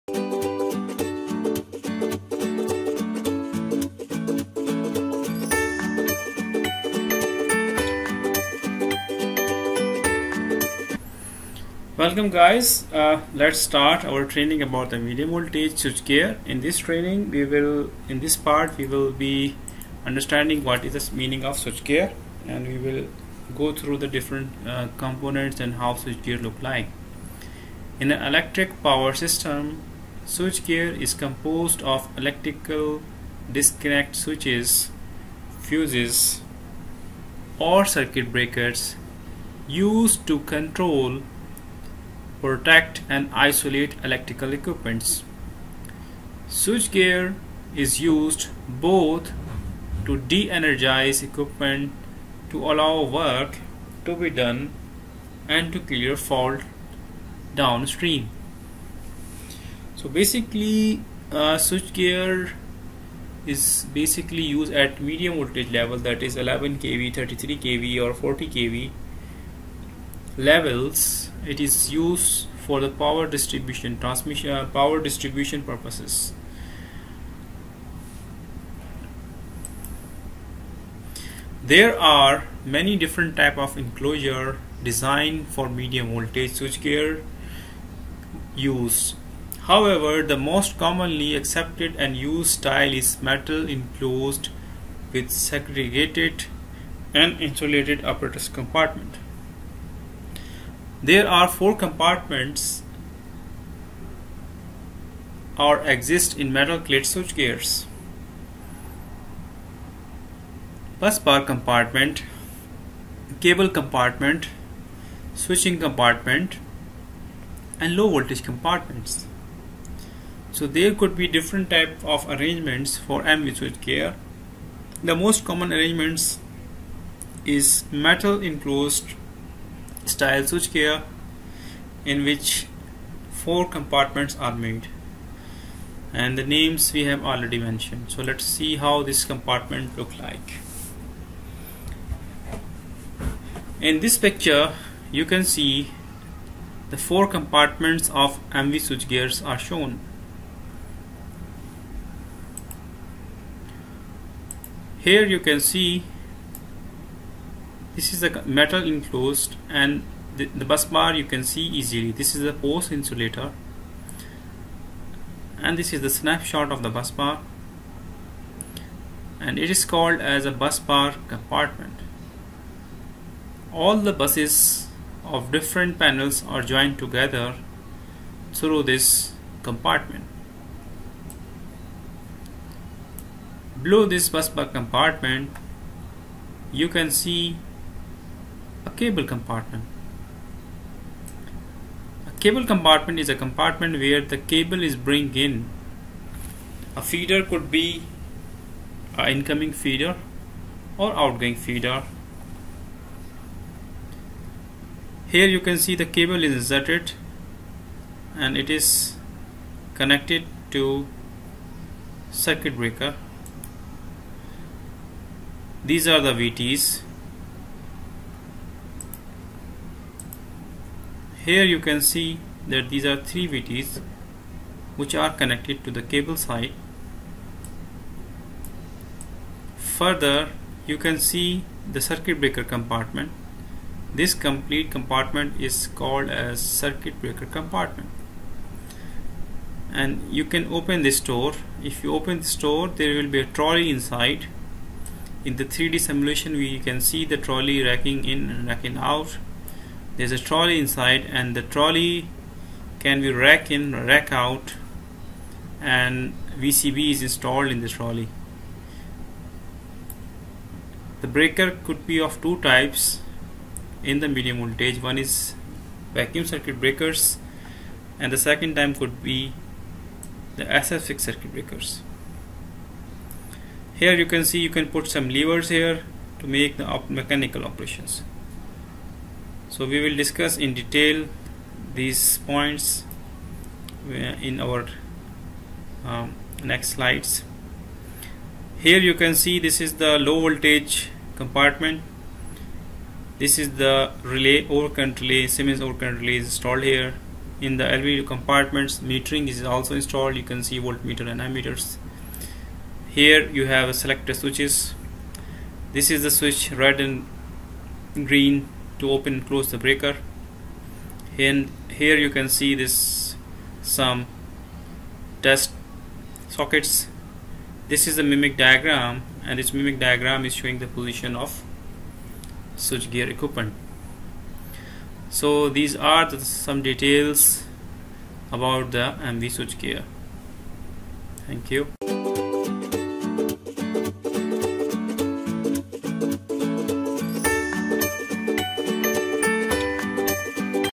MV SWGR Details and definition ( Medium Voltage Switchgear)
Medium Voltage Switchgear
A medium voltage switchgear is a set of electrical equipment enclosed in a predominantly metal structure. This centralized set includes a variety of switches, transformers, fuses, and circuit breakers. Electrical panels are used to better protect, control and separate electrical equipment from each other.
There are many different types of enclosure designs for medium voltage switchgear use. However, the most commonly accepted and used style is metal-enclosed, with segregated and insulated apparatus compartments.
There are four compartments are exist in metal-clad switchgear :
Busbar compartment
Cable compartment
Switching compartment
Low voltage compartment